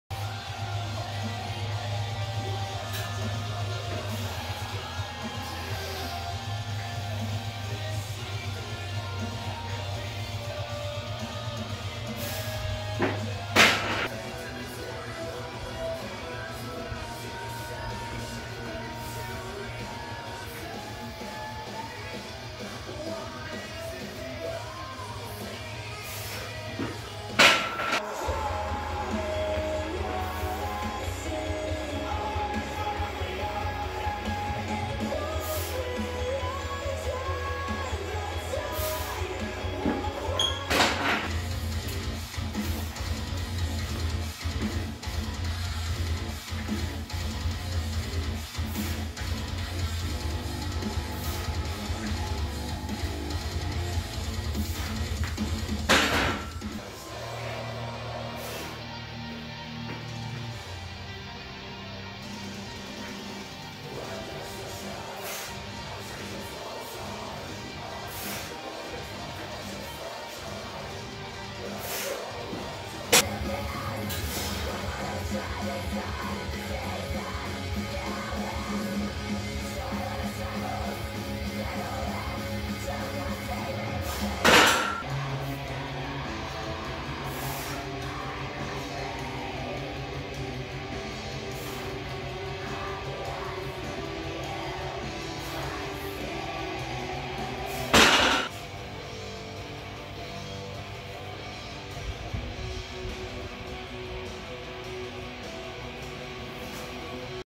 3/27/2020: Squats 335 x 2, Front Squats 265 x 3, Bench Press 225 x 5, Chinups +60 x 3
3/27/2020
Sets 1, 2, and 5 of 5 on Squats
Sets 1 and 3 of 3 on Front Squats
Sets 1 and 6 of 6 on Bench Press
Set 1 of 5 on Chinups